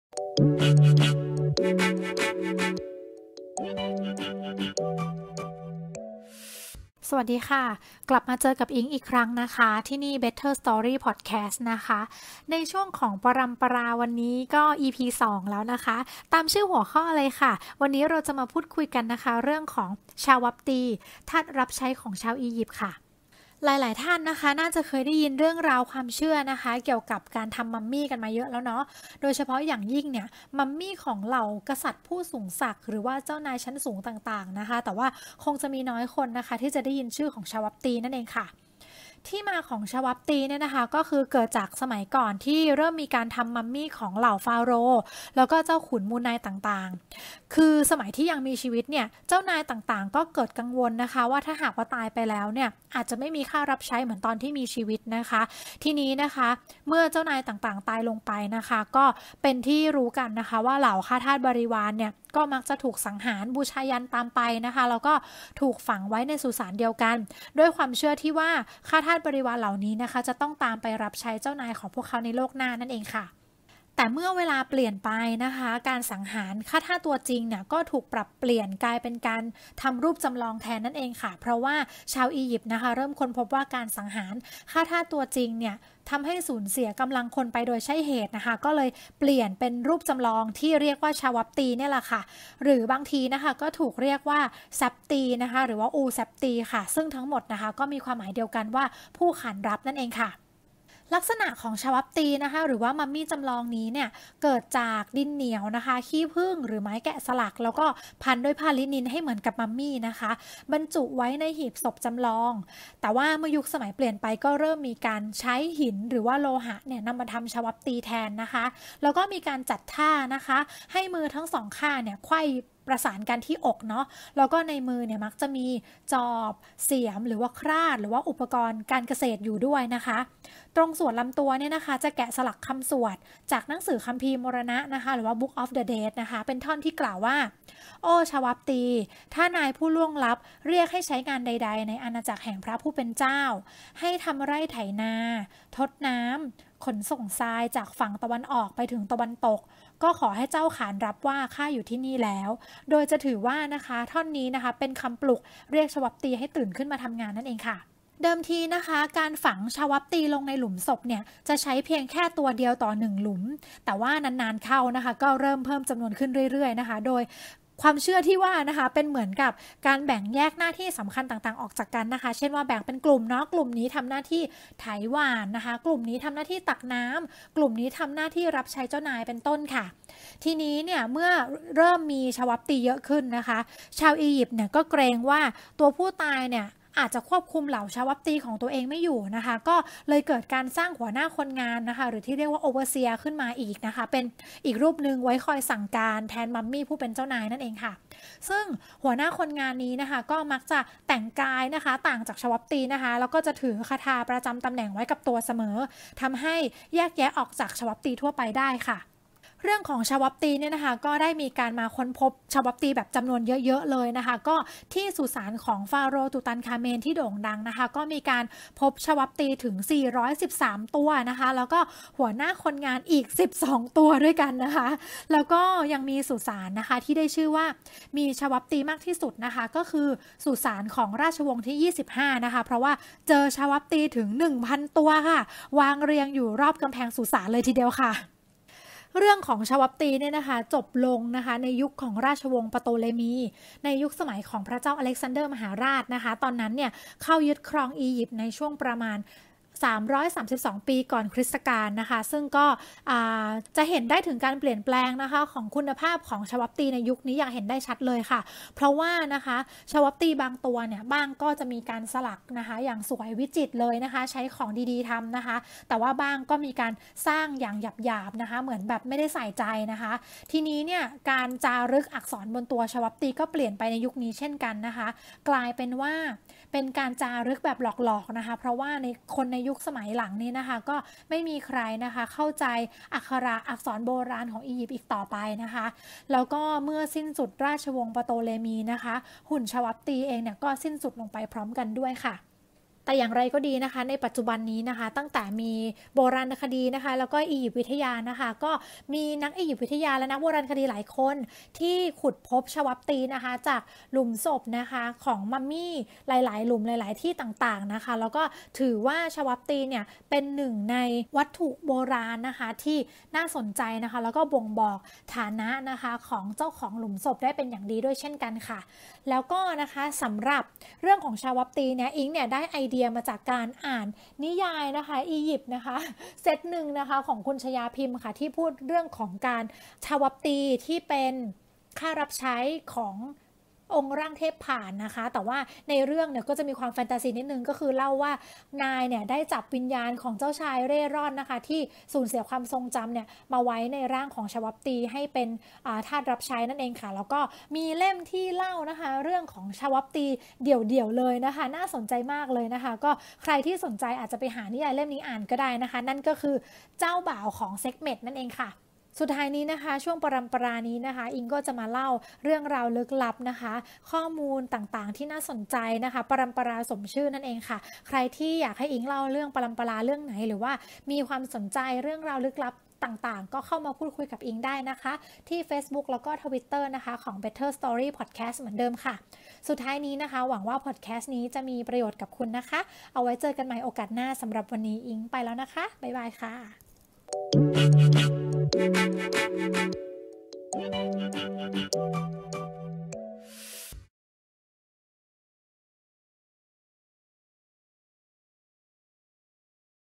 ปรัมปรา EP. 2 ชาวับตี ทาสรับใช้ของชาวอียิปต์
ปรัมปรา EP. 2 เล่าเรื่องเกี่ยวกับ ชาวับตี ทาสรับใช้ของชาวอียิปต์ ที่เชื่อว่าจะตามไปรับใช้เจ้านายจนถึงโลกหน้า จะมีที่มาที่ไปอย่างไร ติดตามฟังได้ในตอนนี้ค่ะ